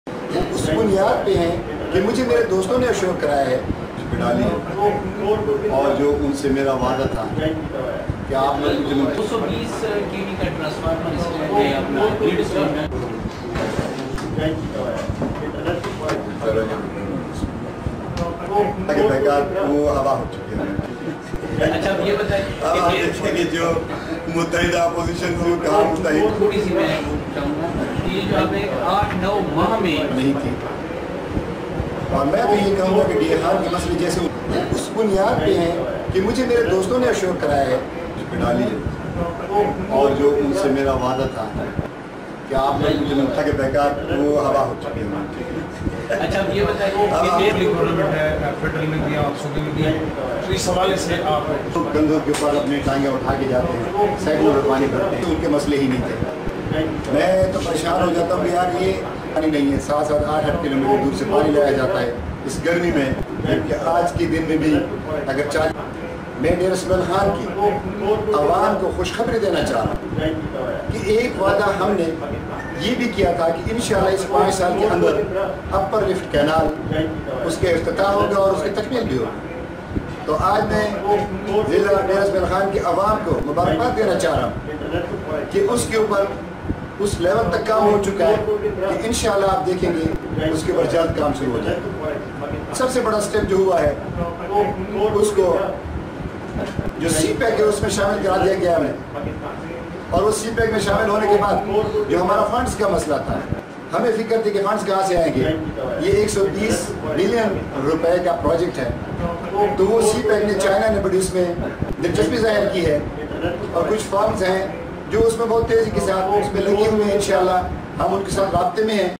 DIKhan: Watch Sheikh Yaqoob MNA PTI NA-39 Press Conference Video on YouTube Uploaded by ApnaDera
DIKhan: Dera Ki Awam Kay Liye Khushkhabri, Left Canal Project Per Kam Shuru, Wazir-e-Azam Imran Khan Jald Mansobay Ka Iftetah Karain Gay, MNA PTI DIKhan-II NA-39 Sheikh Yaqoob Ki Press Conference